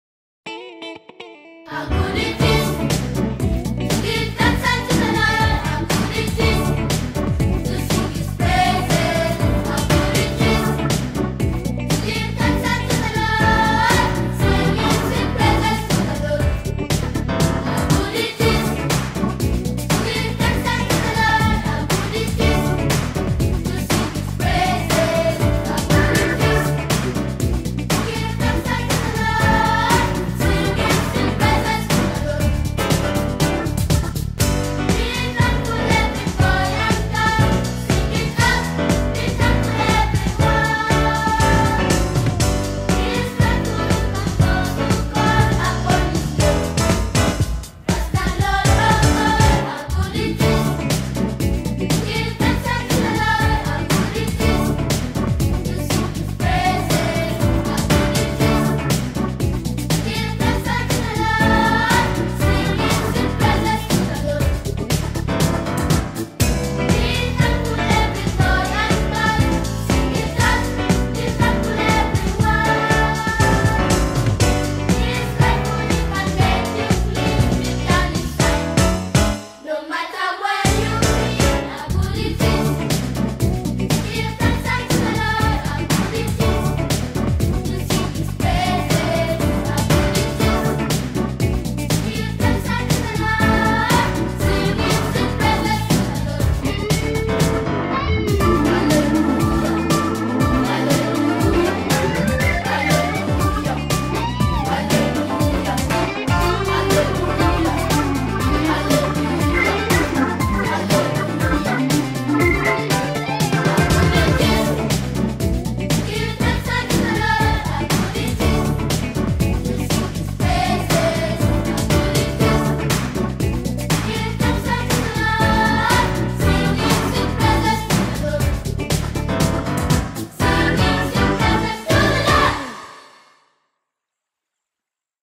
African Children's Choir
We had the African Children's Choir at our church this last Sunday. Unfortunately they would not allow video, but I was able to take pictures and download a song to them :)  They were completely amazing!  Such talent you wouldn't believe.